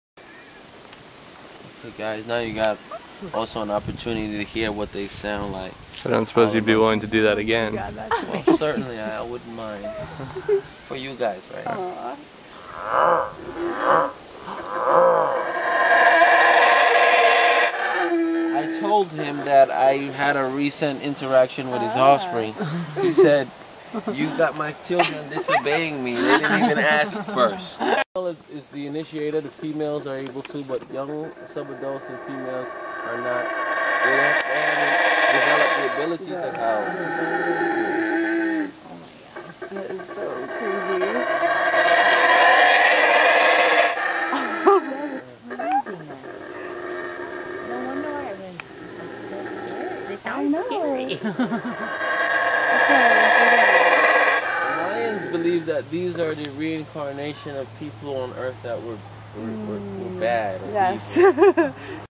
Howling with Monkeys in Belize
Our guide, Shane Baizar, shows us how he communicates with howler monkeys at the Baboon Sanctuary in Bermudian Landing, Belize. Coolest part of my trip. If you are planning a trip to Belize, and can go a bit off the beaten path, contact Shane at ecotourshane(at)yahoo(dot)com